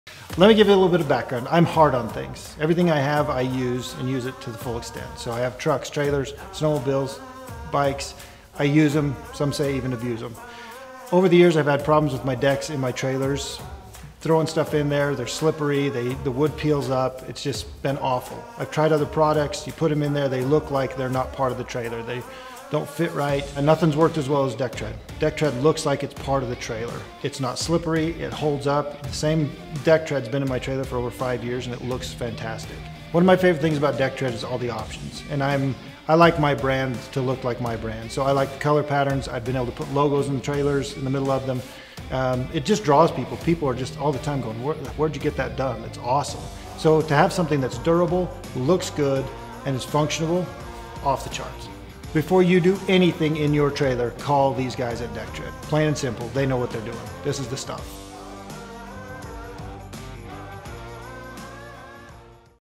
DeckTred Trailer Flooring Customer Testimonial - Dean
Dean shares some of the reason he loves DeckTred in his Trailer.